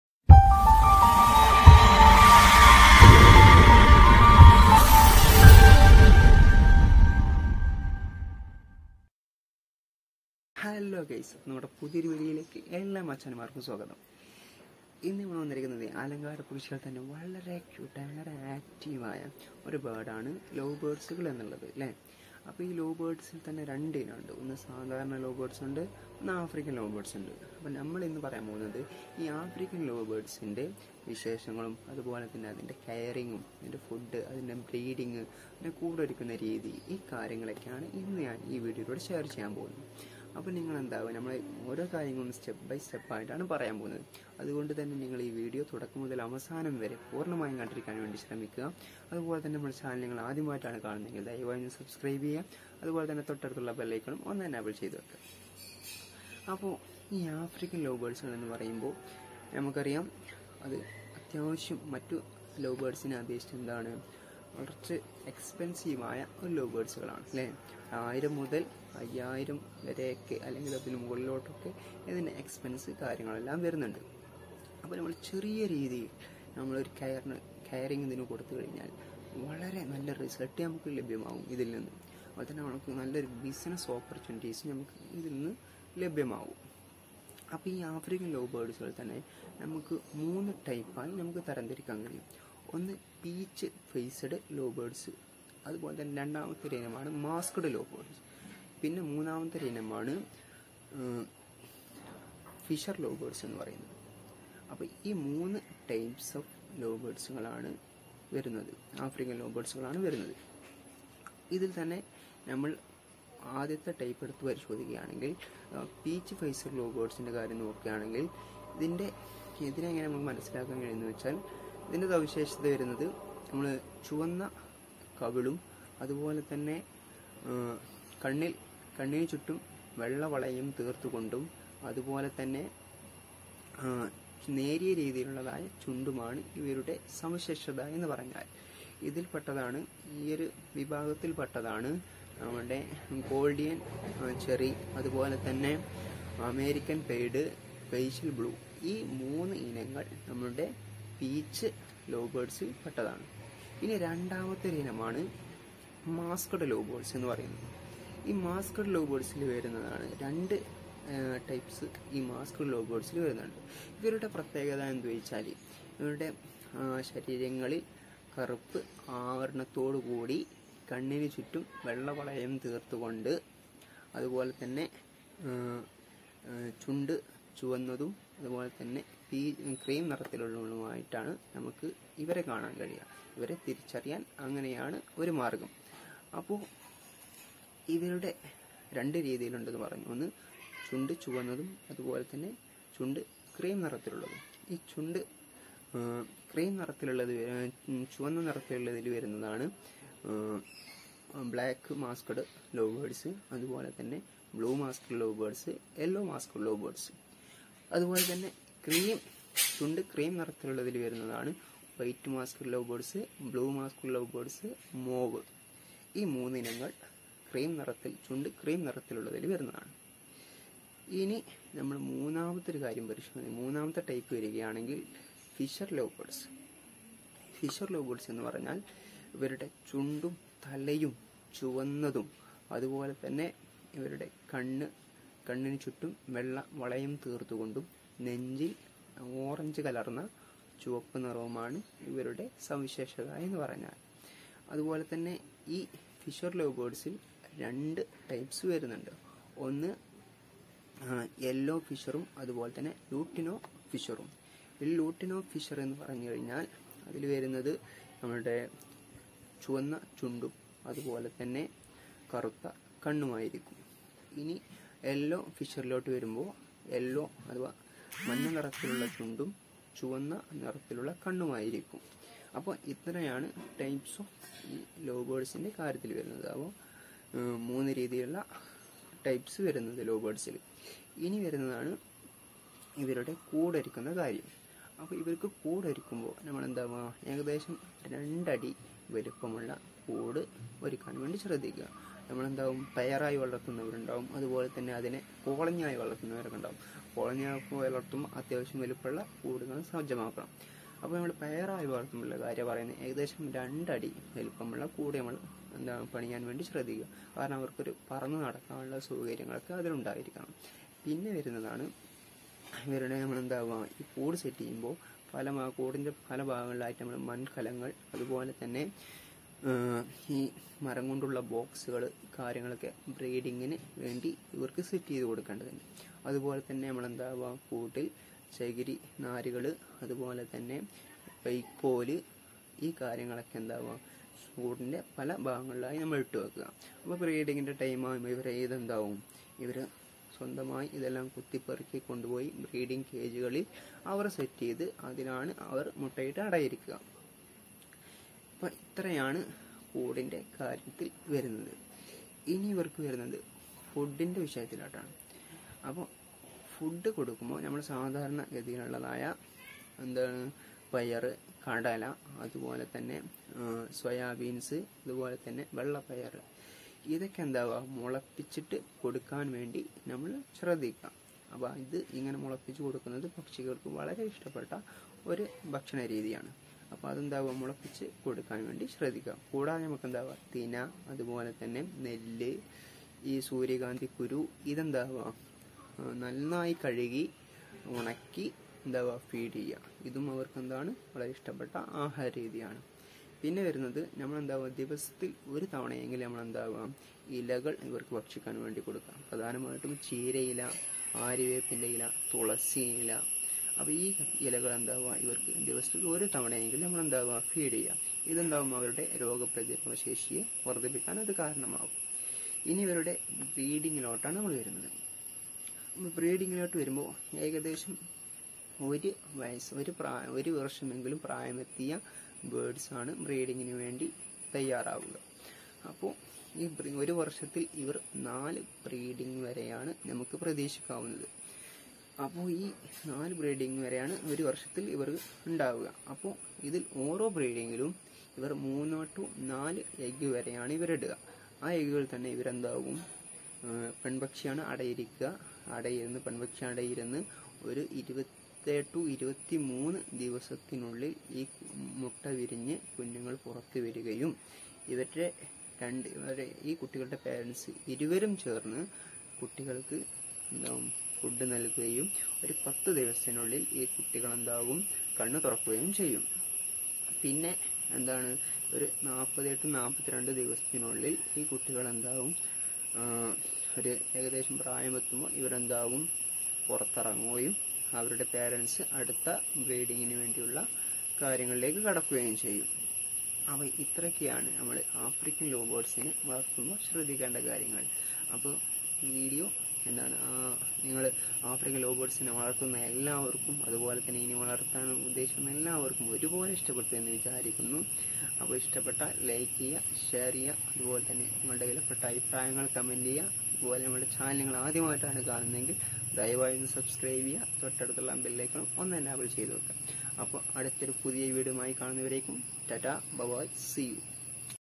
African love birds caring|Feeding Foods|Breeding|Malayalam |Types of african love birds|How to care
African love birds caring, feeding foods, and breeding in malayalam|
Types of african love birds|
All about african love birds|
African love birds v/s normal love birds